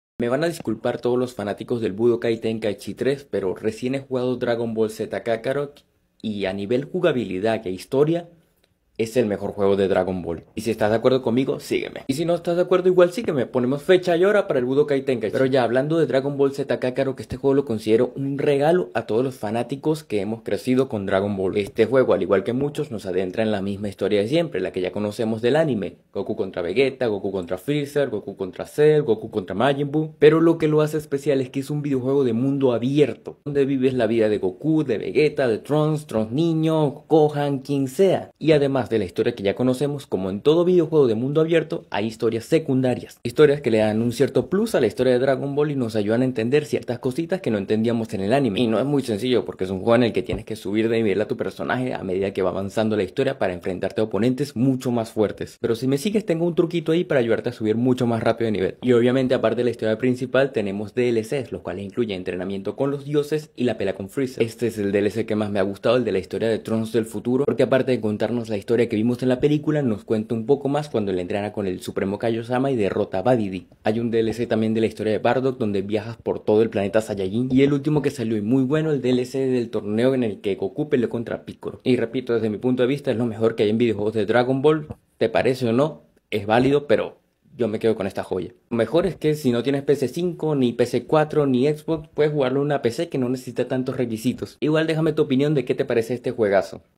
Dragon Ball Z Kakarot: El Veredicto Definitivo
Mi reseña del juego "Dragon Ball Z Kakarot" te presenta el veredicto definitivo sobre este título. Descubre por qué lo considero el pináculo de los juegos de Dragon Ball.